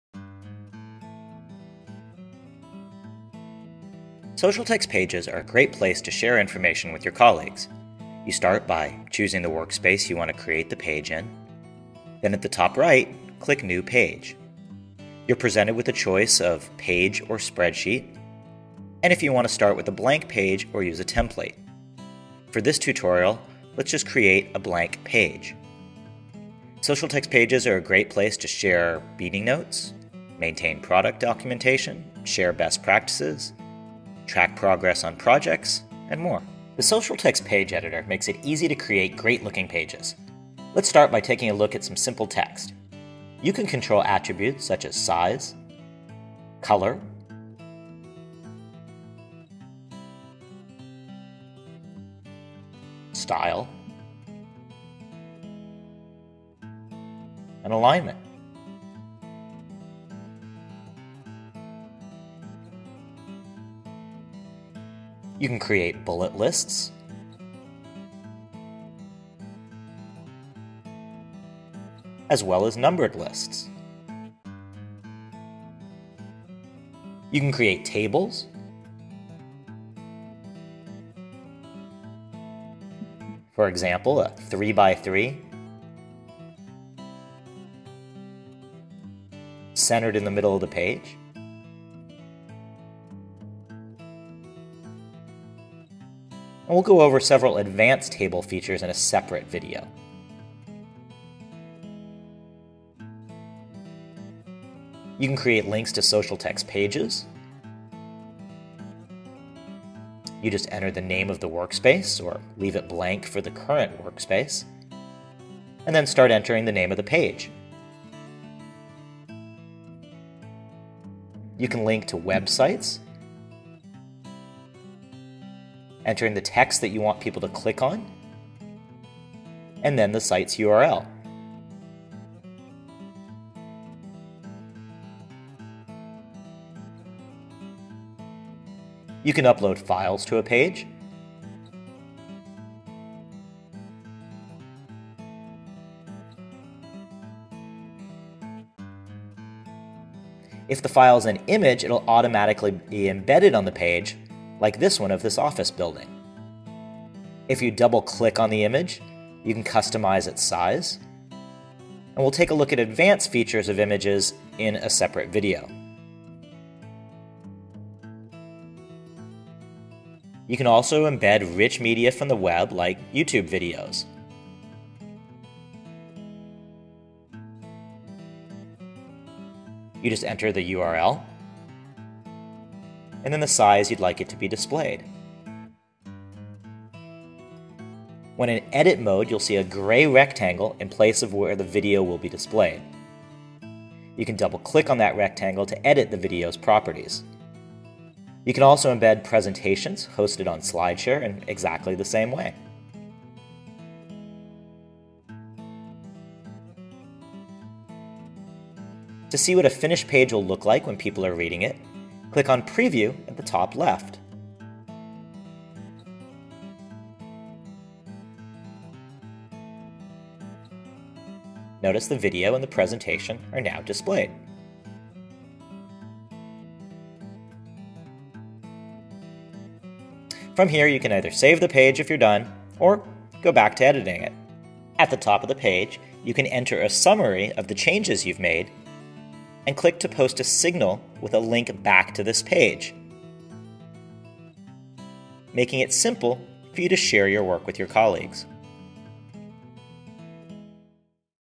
Using The Socialtext Page Editor
Overview of customizing fonts, using tables, adding links and embedding videos.